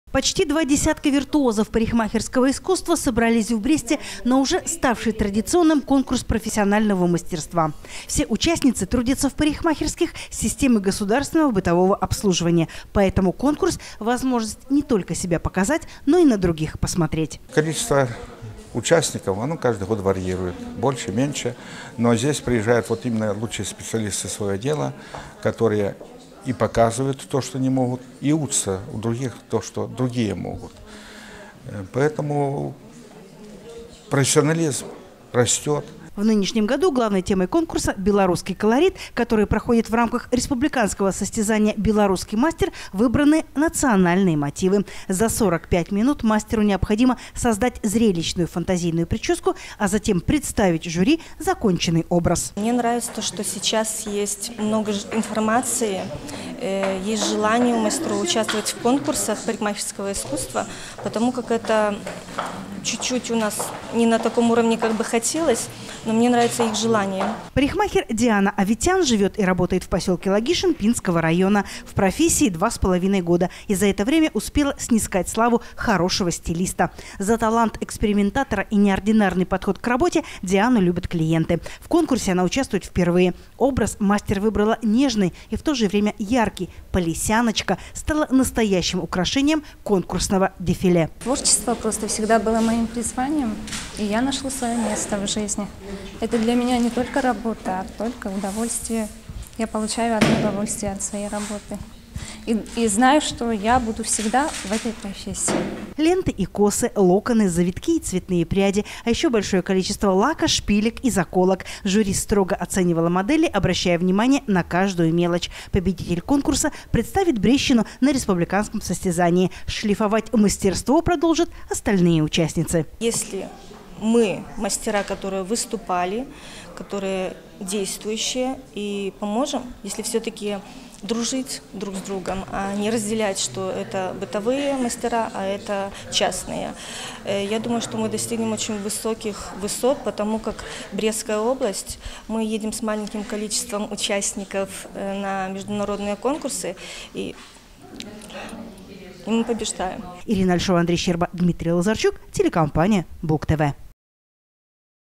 2018-03-14 г. Брест. Областной конкурс профессионального мастерства. Новости на Буг-ТВ.
На базе Брестского колледжа сферы обслуживания прошел областной конкурс профессионального мастерства среди парикмахеров «Белорусский колорит». Организаторами творческого состязания красоты и стиля выступают управление торговли и услуг Брестского облисполкома и областной профсоюз. Поспорить за главный приз и звание победителя самого стильного конкурса собрались мастера практически из всей области. Наш корреспондент Ирина Альшова познакомилась с участниками и понаблюдала за тем, как рождаются новые образы.